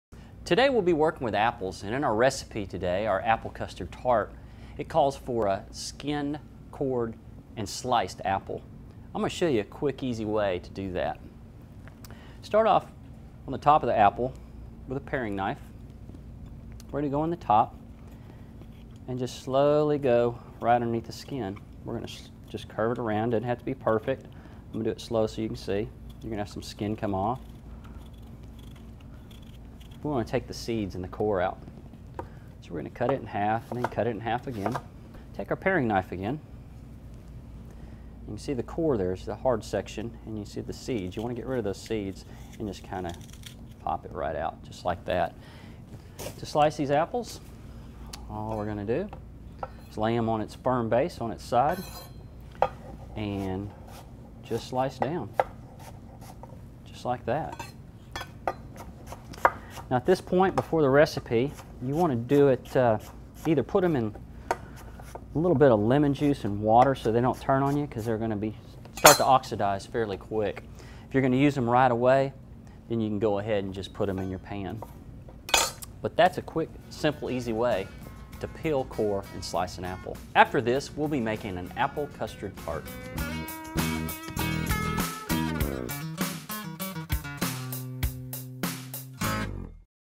Coring and Slicing Apples
Learn from Chef Chris at Market Street about how to prepare apples.